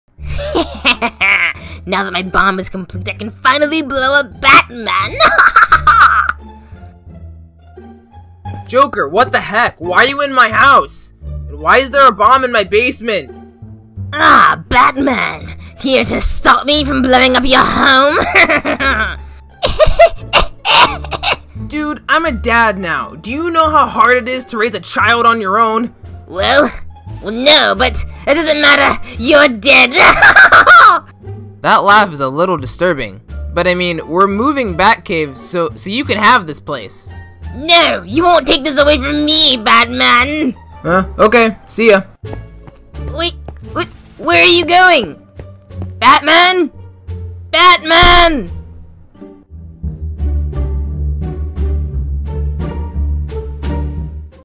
Lego Batman-Failed Plans
The Joker rigs a bomb in Batman's house, little does he know batman is moving!!!